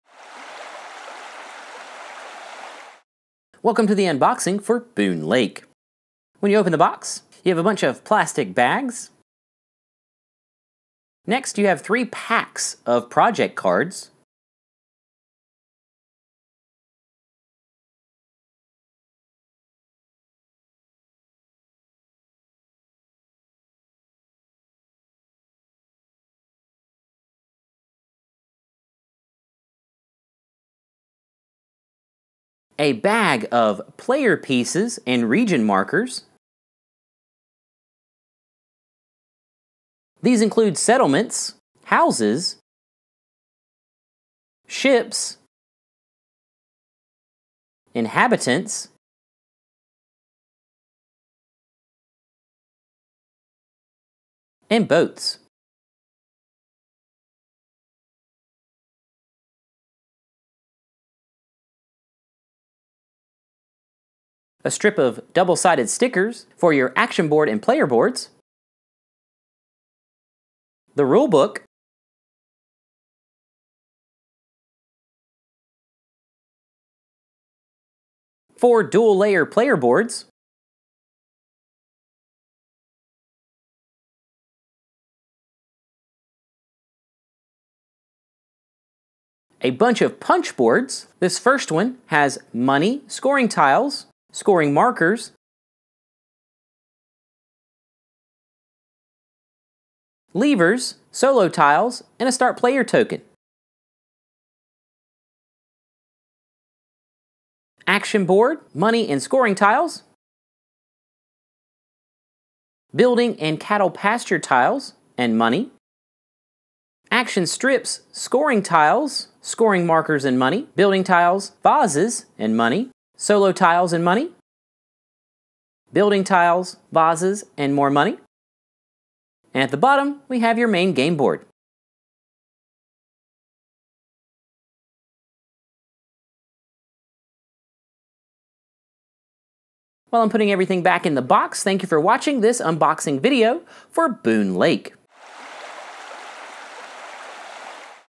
Boonlake: Unboxing
This is a quick unboxing video for the board game Boonlake.